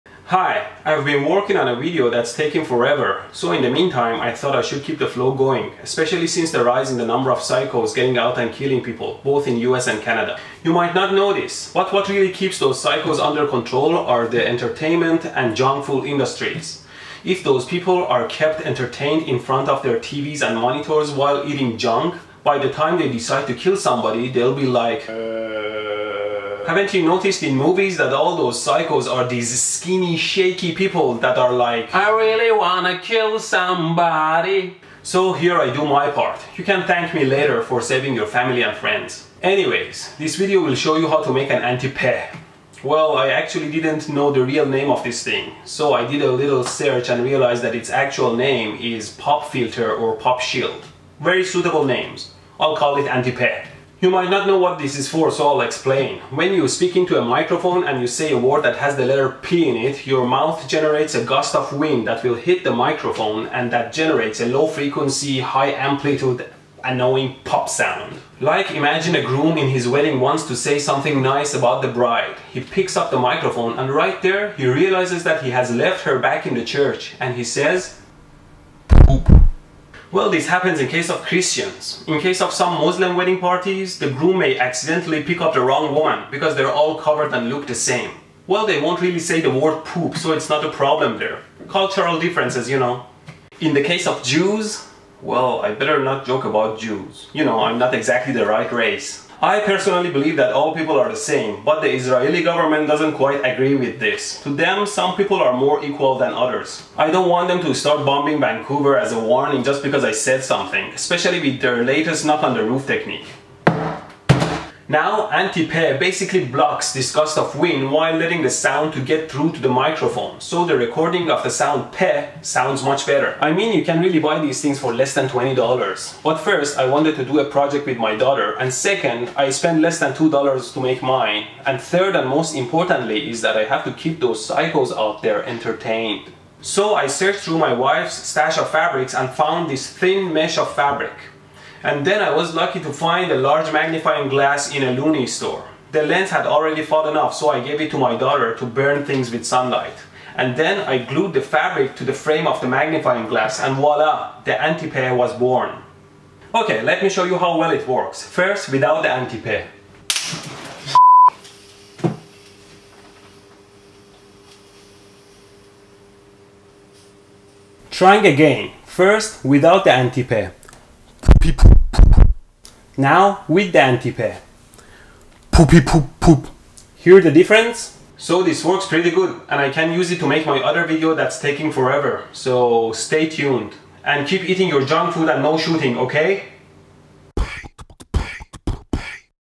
How to Make an "Anti-Peh" (Pop Shield or Pop Filter)
Do you want to avoid annoying pop sounds when recording your beautiful voice? Then learn to make an Anti Peh!